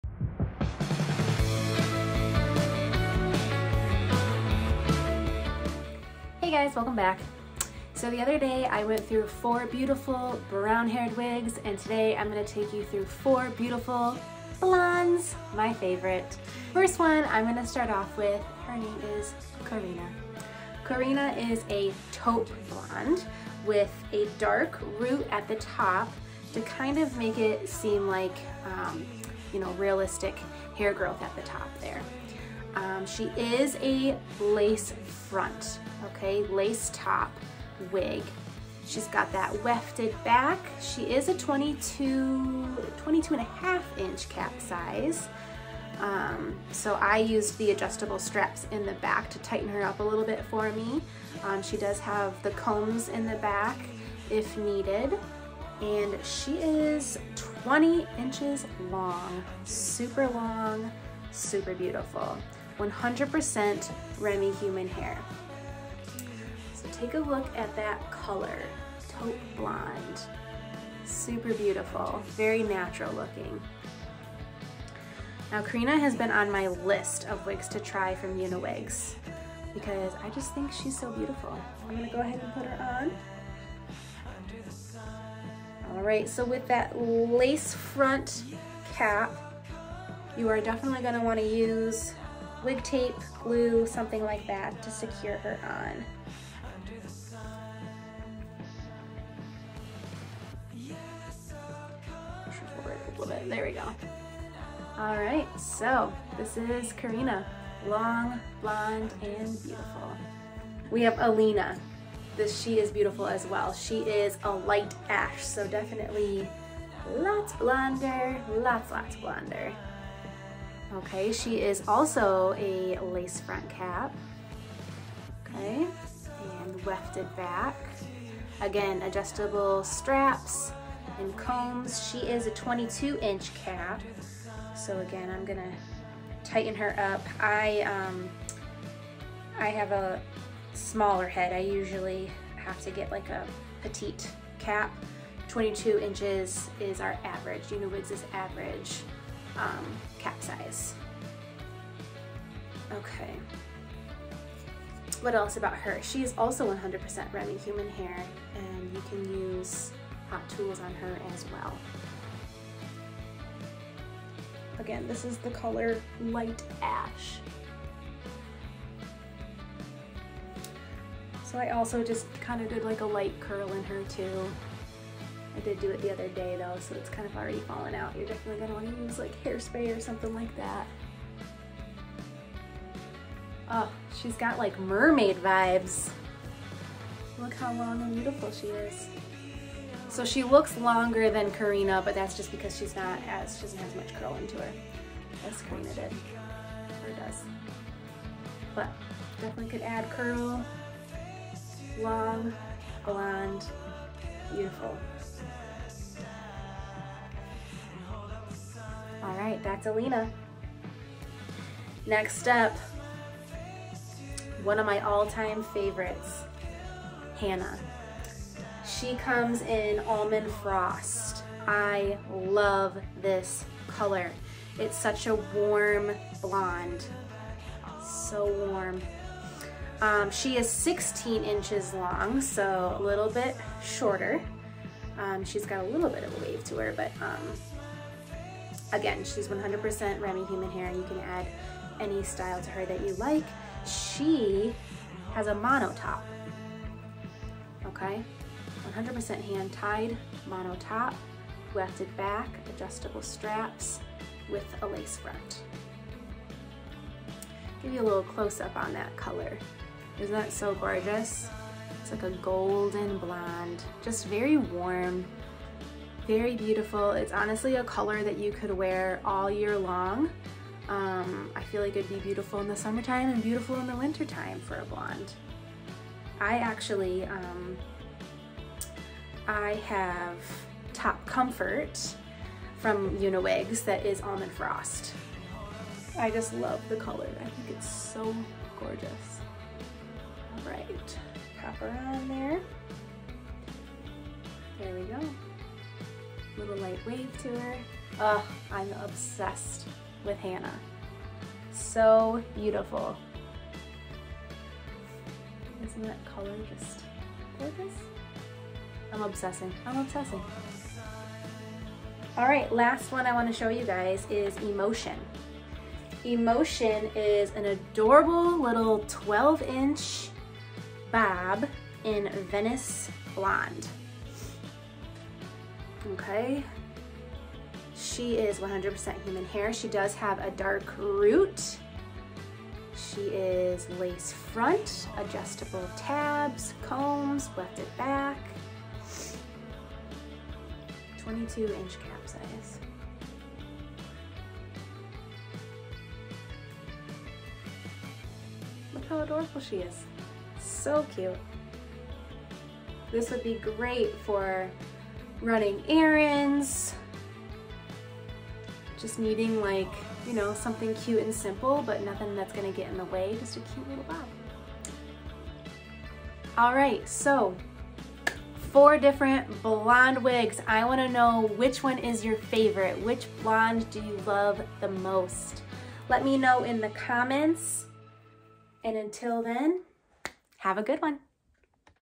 Blondes by Uniwigs
Taking you through 4 beautiful blonde wigs today! Which one is your favorite? Links to each wig is found below.